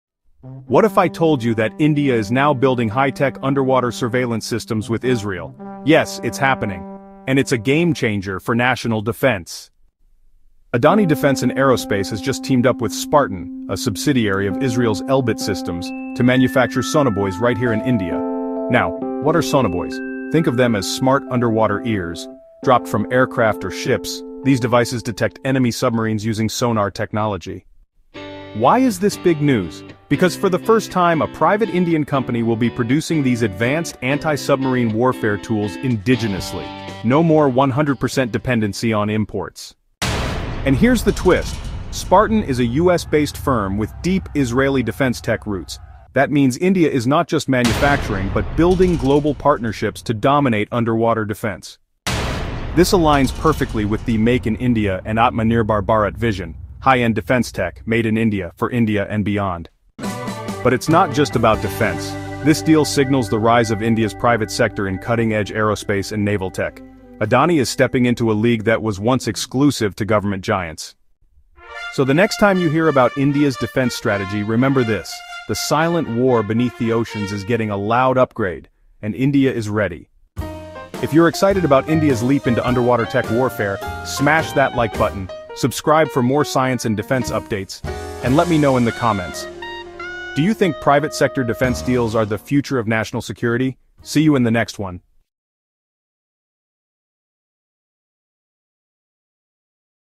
Adani & Israel's Big Defence Deal: Sonobuoys to Boost India's Naval Power | sciX24 Exclusive
**Description (Max \~5000 characters):**

India has taken a giant leap in its defence capabilities with a powerful partnership between **Adani Defence & Aerospace** and **Sparton**, a US-based subsidiary of **Israel’s Elbit Systems**. Together, they will now **manufacture advanced sonobuoys in India**, transforming the landscape of underwater surveillance and anti-submarine warfare (ASW).

Sonobuoys act as the ears of the ocean — dropped from aircraft or warships, these devices are crucial in detecting enemy submarines and securing naval dominance. With this deal, India’s private defence sector is now entering a space once ruled by government organizations.

This move directly supports India's **Aatmanirbhar Bharat** and **Make in India** initiatives, while building deep international defence collaboration with Israel and the US. It also marks the first time an Indian private company will offer indigenously developed sonobuoy systems to both domestic and global markets.

In this video, we explain:

* What sonobuoys are and how they work
* Why this Adani-Sparton deal is a big win for India
* How this move strengthens India's Undersea Domain Awareness (UDA)
* The strategic implications for Indian Navy and regional security
* What this means for the future of private defence manufacturing in India

**Tags (max 500 characters, no repetitions):**